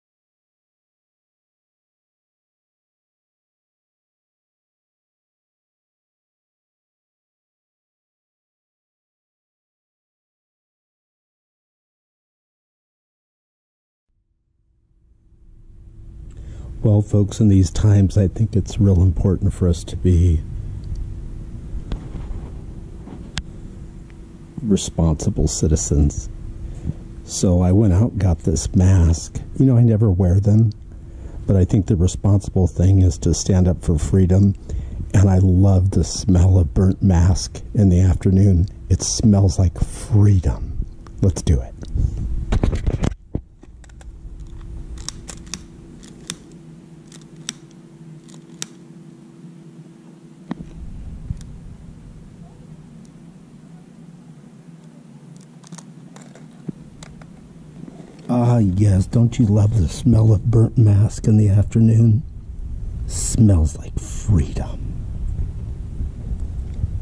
Smells like Freedom
Youtube wants to hide this video from you as they banned it but here it is look at this
Conservative Christian American man with the lighter here below in debate